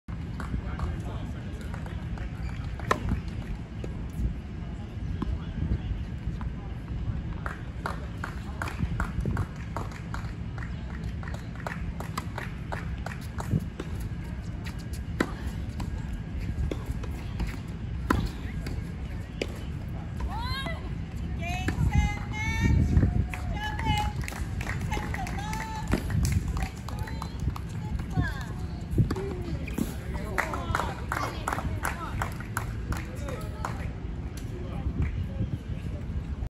Match point 2021 G14s Junior Orange Bowl final
Iva Jovic defeated Shannon Lam 6-3, 6-0 in G14s Junior Orange Bowl final, Biltmore Tennis Center, Coral Gables Florida, December 21, 2021.